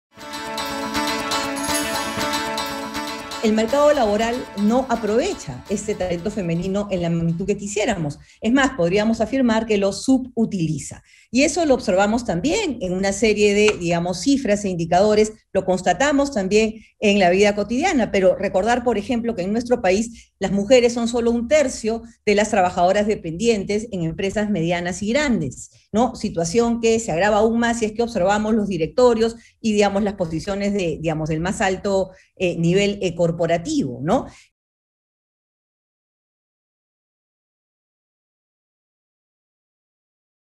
Norma Correa, investigadora
El mercado laboral no aprovecha este talento femenino en la magnitud que quisiéramos. Es más, podríamos afirmar que los subutiliza.